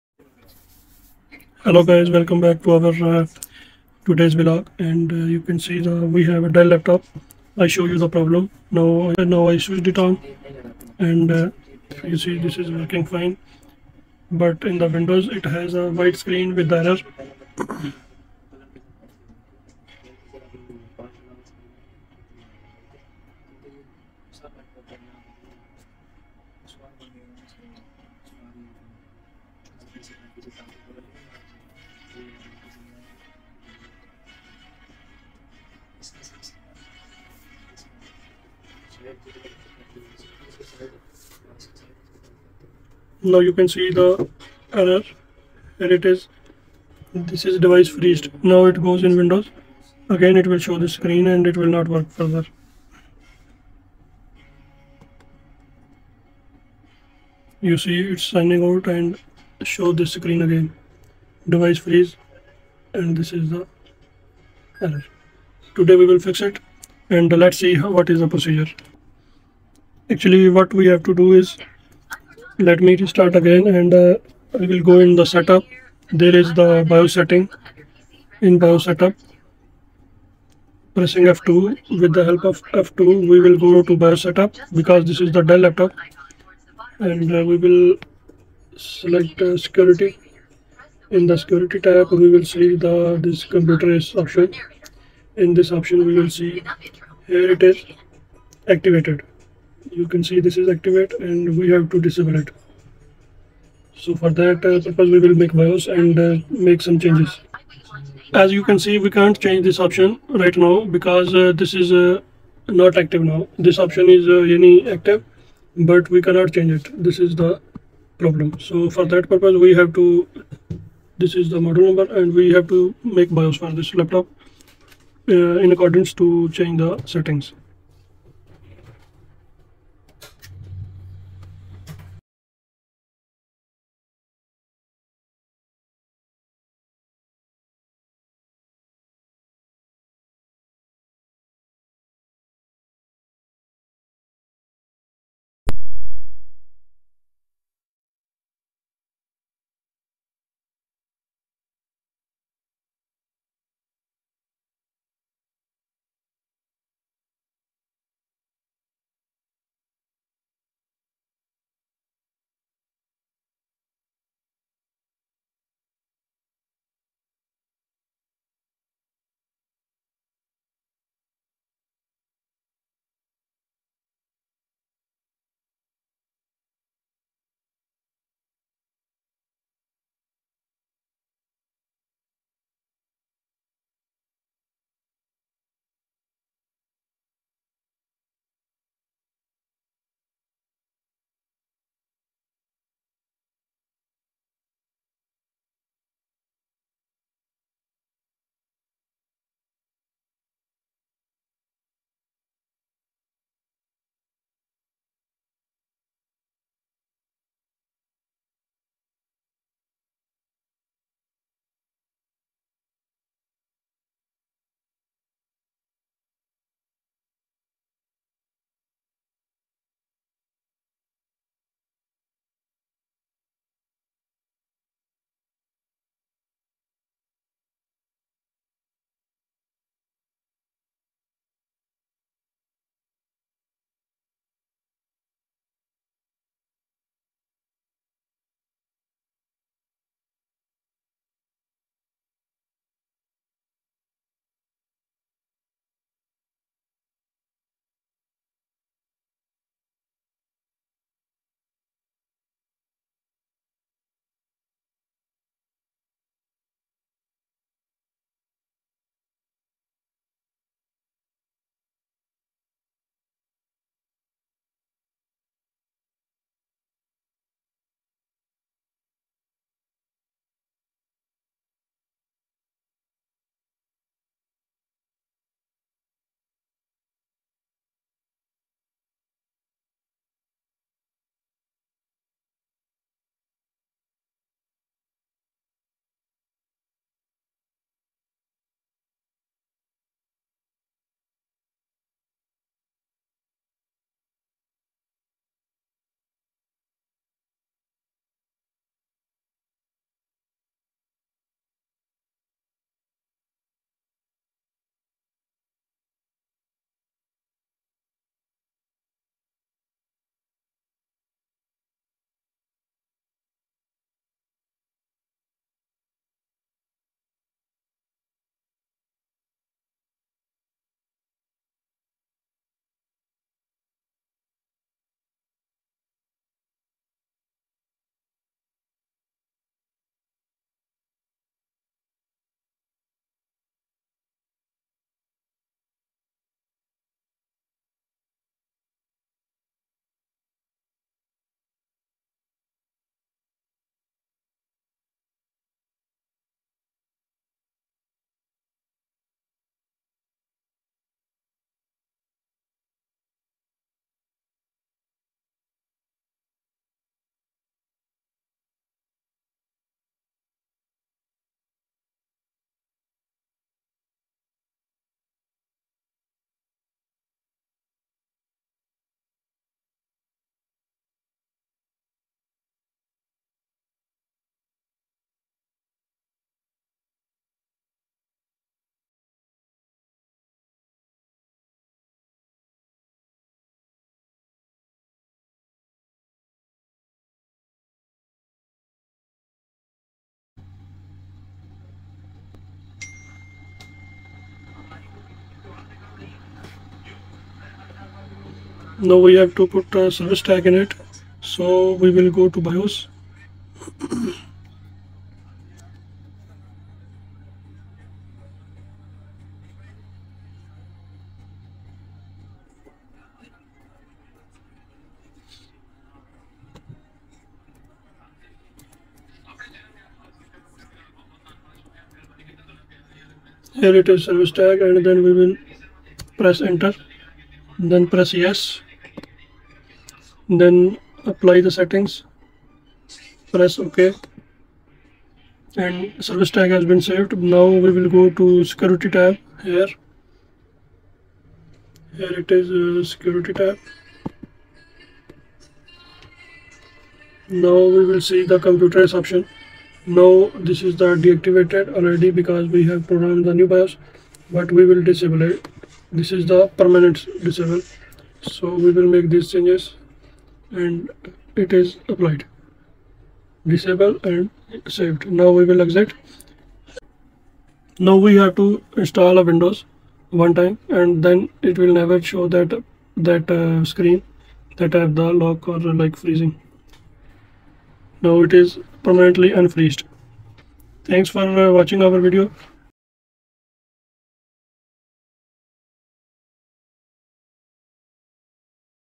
how to fix laptop freeze. school lock
in this video, we are unlocking school lock for learning purpose. we have dell windows laptop school locked.